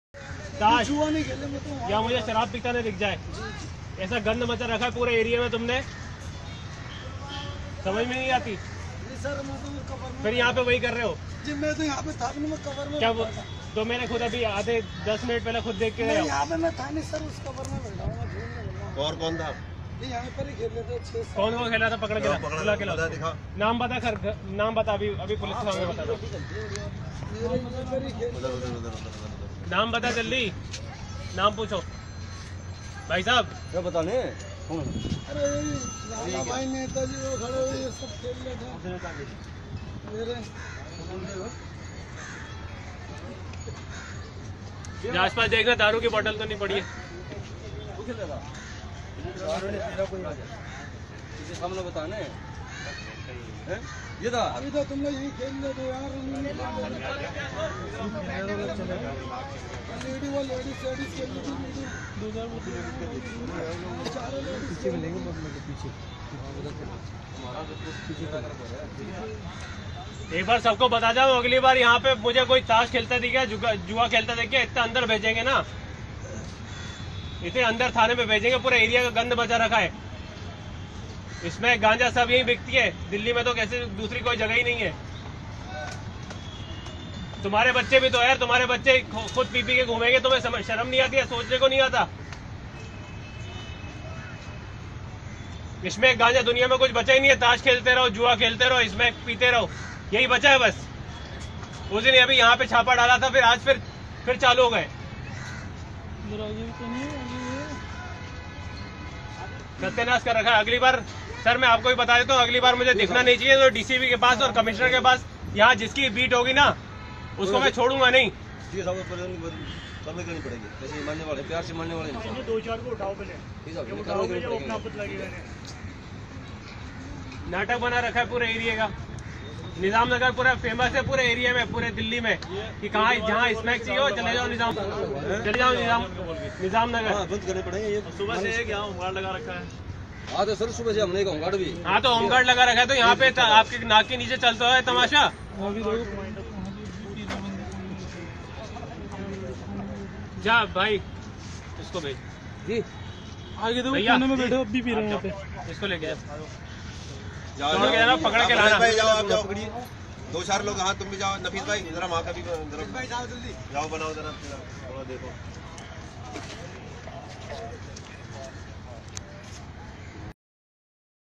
praveen kumar mla delhi जंगपुरा विधानसभा में नशाखोरी बर्दाश्त नहीं की जाएगी,किसी को बक्शा नहीं जाएगा
praveen kumar mla delhi